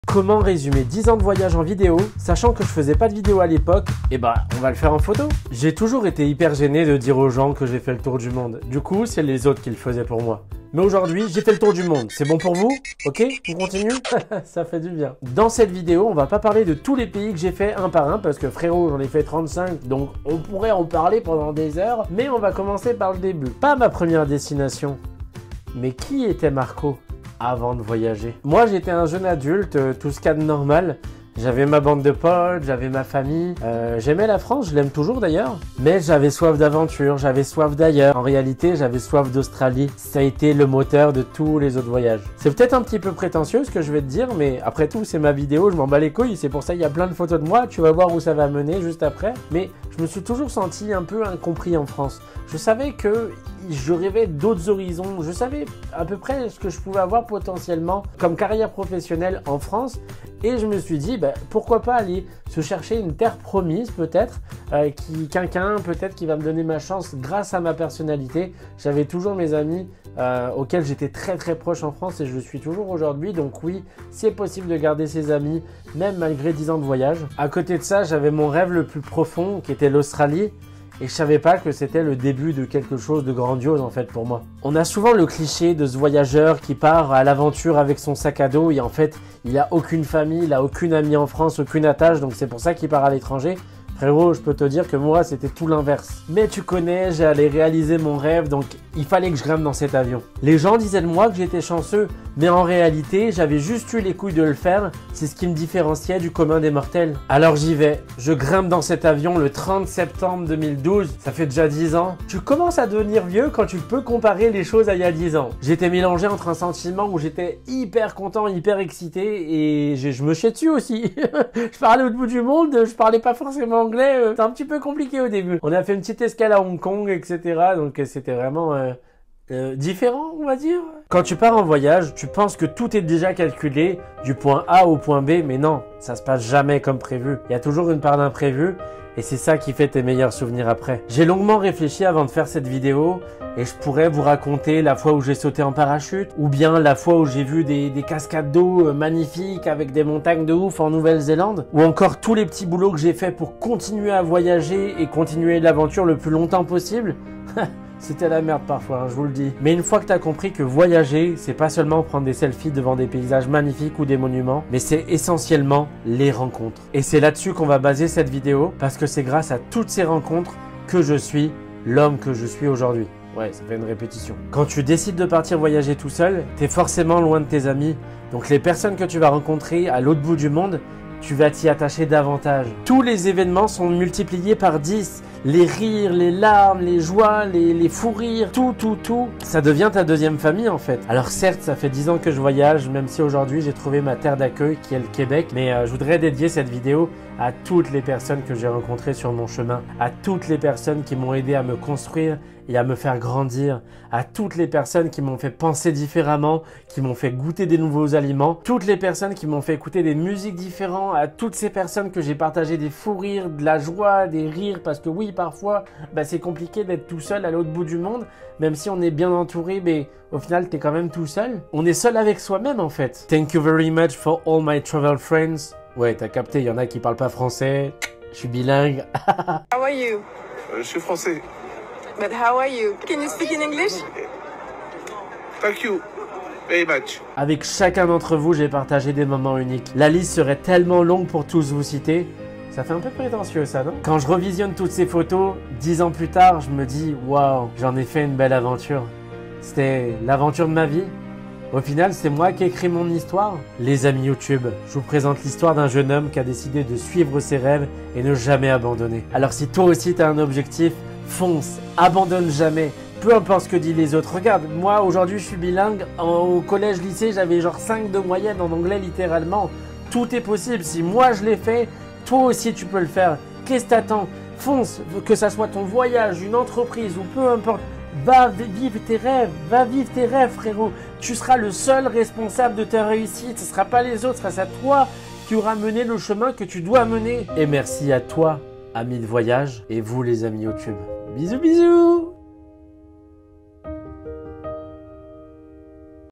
10 ANS DE VOYAGE 🌍
J'ai exploré notre belle planète pendant 10ans a faire le tour du monde, aujourd'hui je voulais vous partager cette merveilleuse aventure en vidéo.
Je pense que c'est la vidéo la plus personnel de ma chaine au montant dans cette avion il y a 10ans j'étais loin de m'iamginer de vivre tout cela et ensuite le raconter sur YouTube devant des milliers de personnes.
Je me suis découvert lors de mes voyage en tant que jeune adulte mon aventure a d'abord commencer pars l'Australie et l'assomption de 34 autres pays.
Merci a tout les amis de voyage croisé sur ma route c'est grâce a vous que je suis l'homme que je suis aujourd'hui.

RÉSEAUX POULOULOU!

Commente " Tour du monde " si tu as lu la description en entier